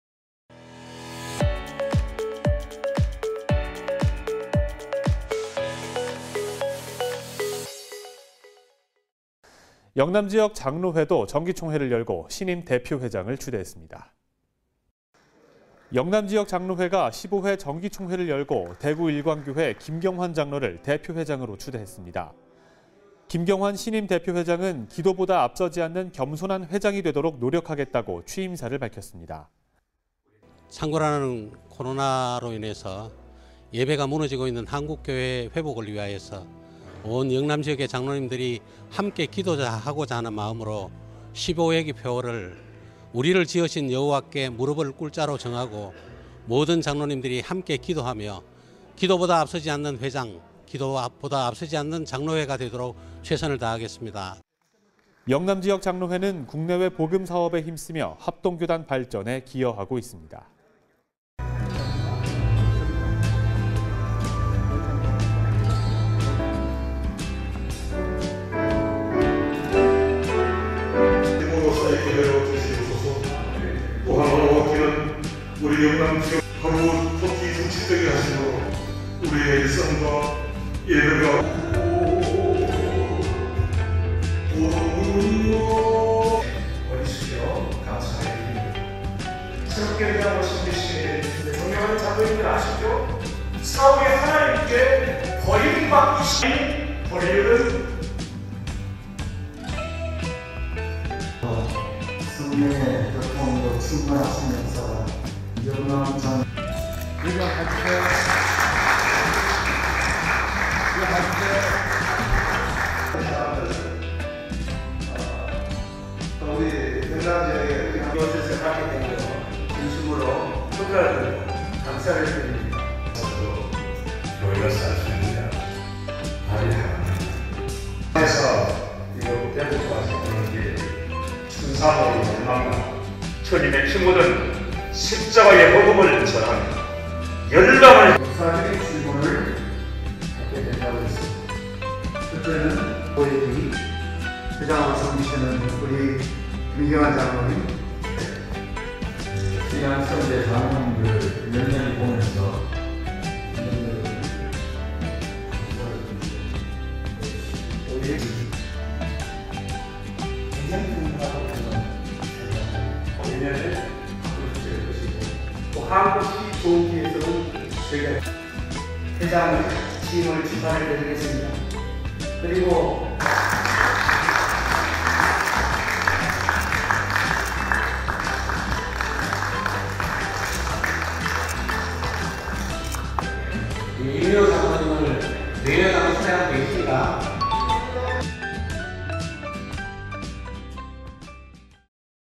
영남지역장로회 제15회 정기총회 개최(4월4일 CBS뉴스)
영남지역장로회가 제15회 정기총회를 3월 31(목) 오전 10시 30분 푸른초장교회에서 열고 김경환장로(대구일광교회)를 대표회장으로 추대했다.

1부 예배는 이민호장로(대표회장)의 인도로 박성규목사(부전교회)가 행13:22 말씀을 가지고 '동행과 바싹 따라 감'이라는 제목의 메시지를 전했다.

박 목사는 "다윗이 사울과 달랐던 점은 하나님의 마음에 합한 자 이었던 것"이라며, "하나님과 동행한다는 것은 하나님을 존중하며 경외하는 삶이다. 하나님을 존중하며 경외하며 사역하는 영남지역 장로회가 되길 소망한다"고 당부의 말씀을 전했다.

이어 2부 격려 및 축하 시간은 김경환장로(수석부회장)의 사회로 권순웅목사(예장합동 부총회장)의 격려사, 오정호목사(한국교회봉사단 공동대표), 허은목사(총회서기), 강진상목사(전국영남교직자협의회 대표회장), 김형국원로목사(하양교회), 강의창장로(증경부총회장), 윤선율장로(증경부총회장), 오광춘장로(호남지역장로회연합회 회장), 임영식장로(대신대학교 재단이사장)의 축사가 진행되었으며 미자립교회 및 선교사에게 후원하는 시간도 이어졌다.

3부 정기총회에서는 지난 1년간 진행한 사업보고와 재정보고 등을 동의와 재청의 절차를 거쳐 통과하고 신임원 개선을 통해 김경환장로(대구일광교회)를 제15회기 영남장로회 회장으로 추대했다.

영남지역장로회는 영남지역 장로들의 믿음 생활 및 국내와 해외 복음사업에 힘쓰며 전국 연합회와 지역장로회 회원 상호간 친교를 통해 합동교단 발전에 기여하는 장로회이다.

[인터뷰] 김경환장로(대구일광교회)/영남지역장로회 대표회장

창궐하는 코로나로 인해서 예배가 무너지고 있는 한국교회 회복을 위하여서 온 영남지역 장로님들이 함께 기도하고자 하는 마음으로 15회기 표어를 '우리를 지으신 여호와께 무릎을 꿇자'로 정하고 모든 장로님들이 함께 기도하며, 기도보다 앞서지 않는 회장, 기도보다 앞서지 않는 장로회가 되도록 최선을 다하겠습니다.